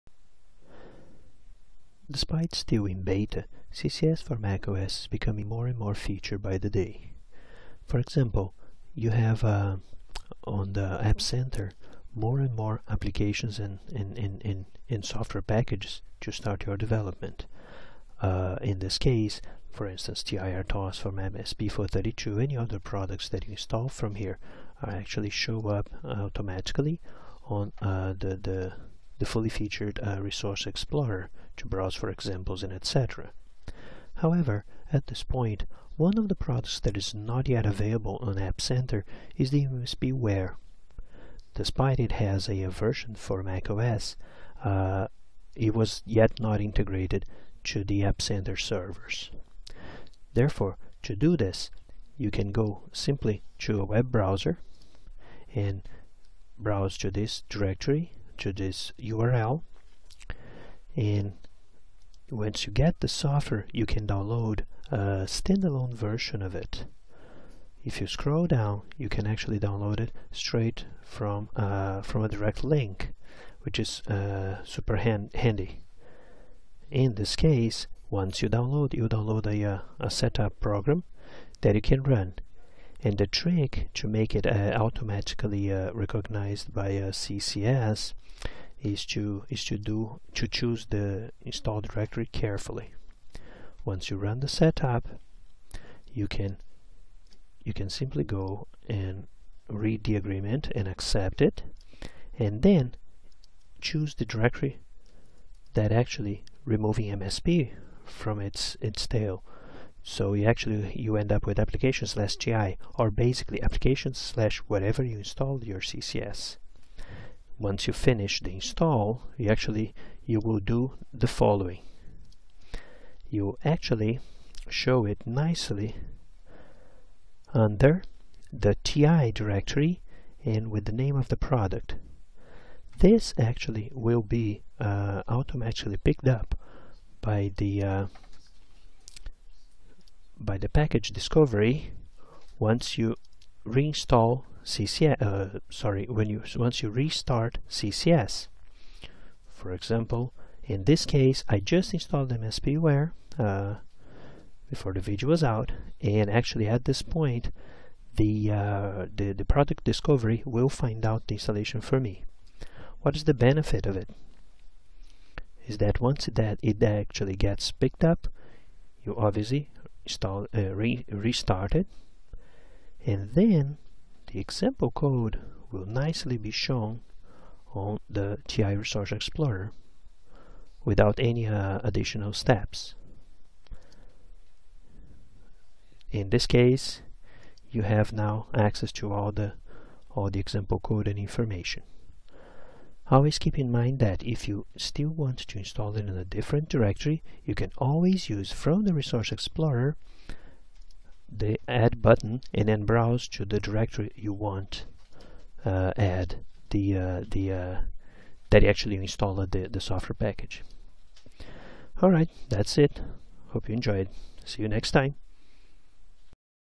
How to properly add offline installed products to CCS
How to add products to CCS and have their contents automatically added to the Resource Explorer via the product discovery tool.

This quick tip uses the offline MSPWare installer on CCS for Mac OS X.

Links:
MSPWare: